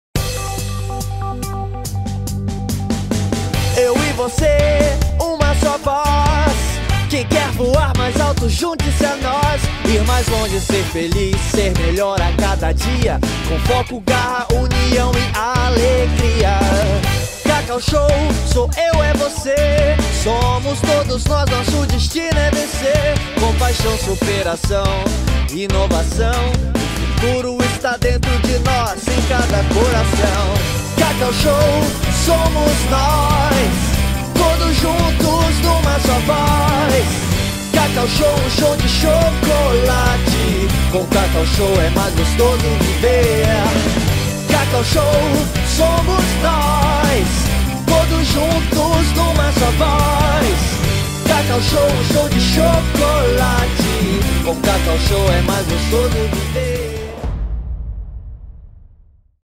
Mega CacauShow Parkshopping Campo Grande RJ - Videopromo
Videopromo desenvolvido para a nova Mega Cacau Show do Parkshopping Campo Grande no Rio de Janeiro.